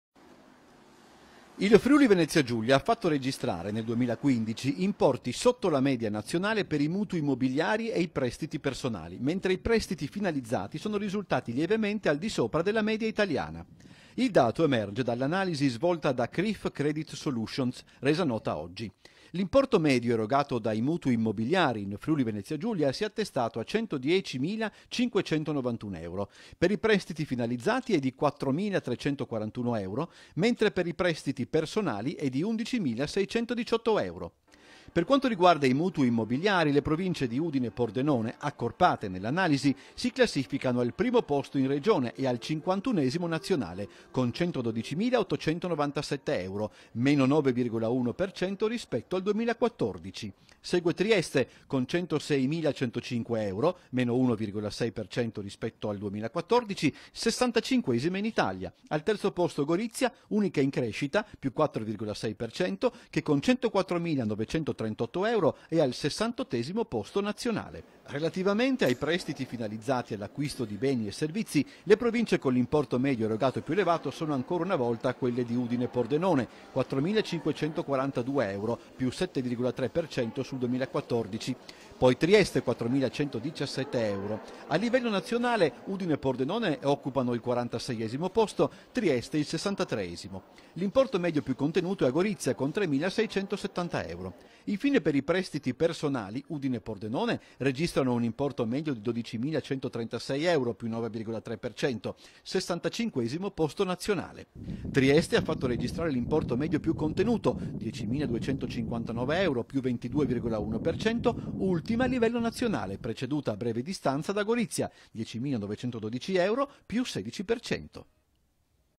27/04/2016 - CREDITO, FVG SOTTO LA MEDIA NAZIONALE
IL NOTIZIARIO (mercoledì 27 aprile 2016) - In Friuli Venezia Giulia, i mutui casa e i prestiti personali risultano al di sotto della media nazionale.